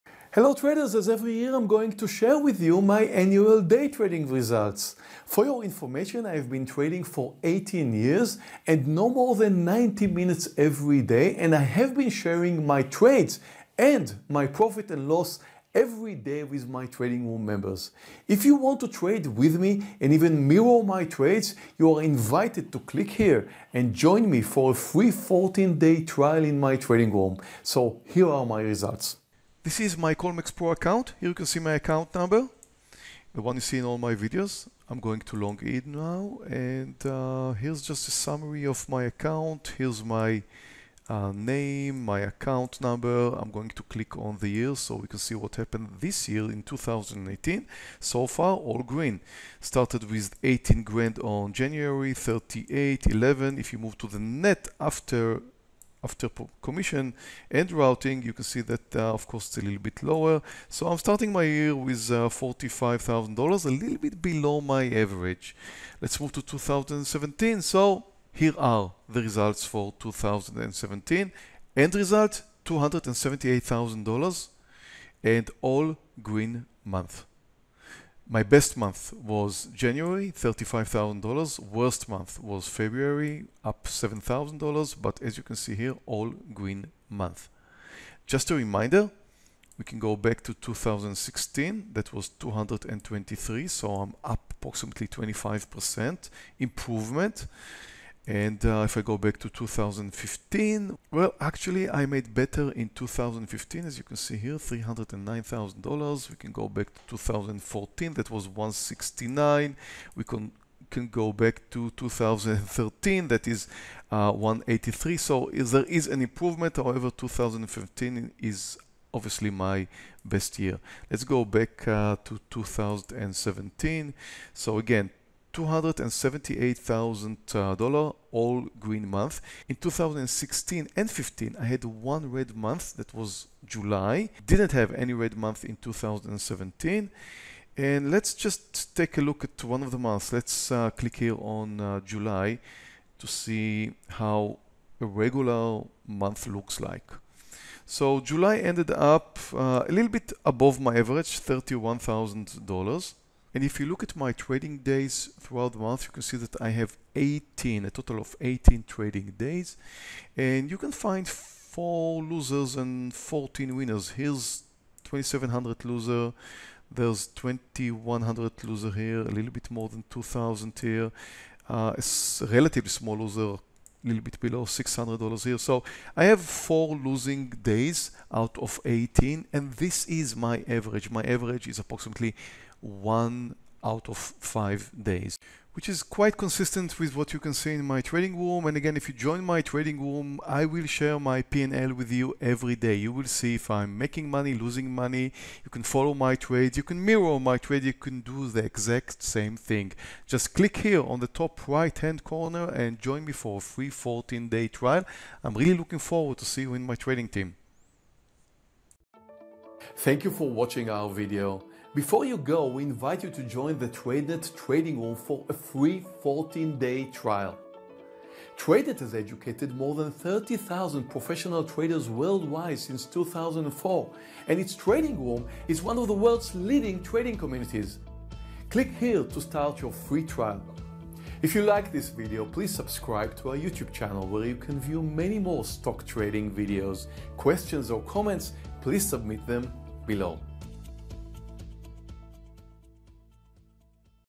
Day Trading Results 2017 - Meir Barak
In this video you will see my day trading Colmex account profit and loss for 2017 and some previous years. All my trades were taken in my live trading room and my P&L is posted every single day. In this video you can see the summery of my annual activity. you are welcome to join my trading room for a free 14-day trial